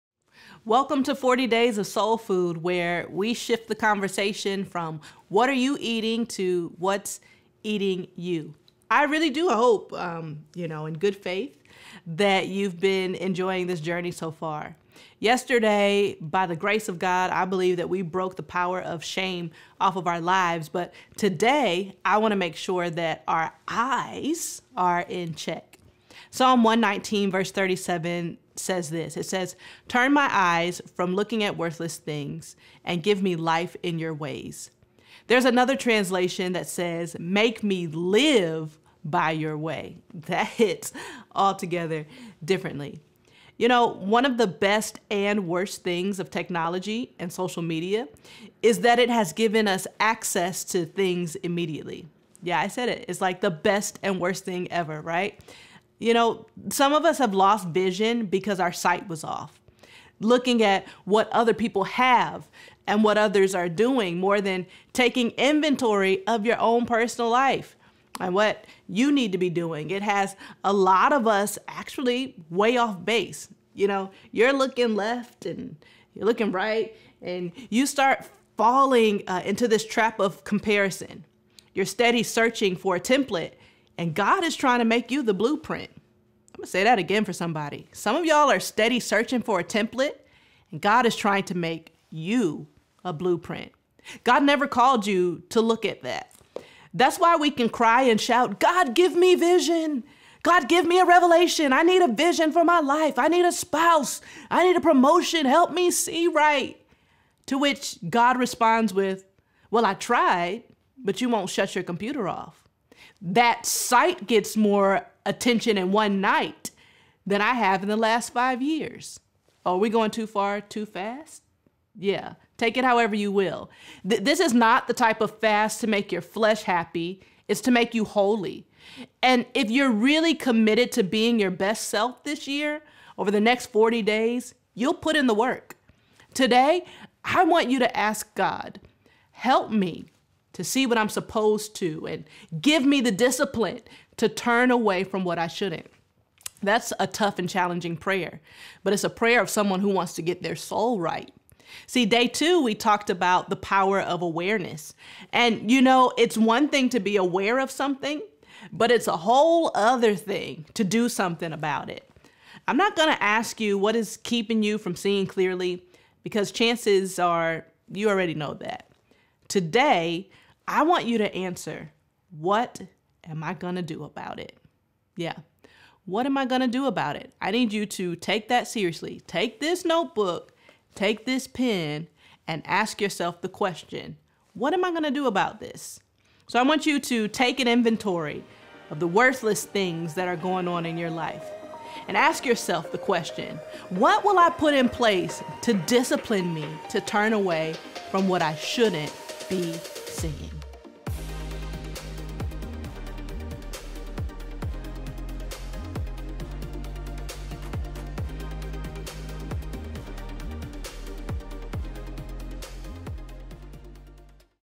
Day 4 - Restore my Sight
Day 4 📓
Join Dr. Lesley and C3 Hampton as we embark upon a 40 day soul fast. For the next 40 days we will have daily videos centered around releasing toxic people and ideas, renewing our minds, and restoring our health. Remember, it’s not just about what you are eating but what’s eating you!
➡️ Get your journal and pen ready.